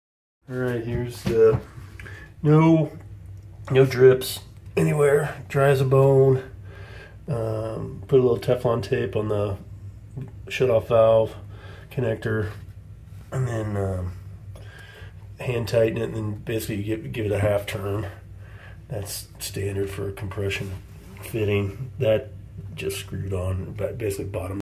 Vintage American Standard Compact ballcock valve replacement(1)
Replace possibly original ballcock valve in vintage American Standard Compact toiley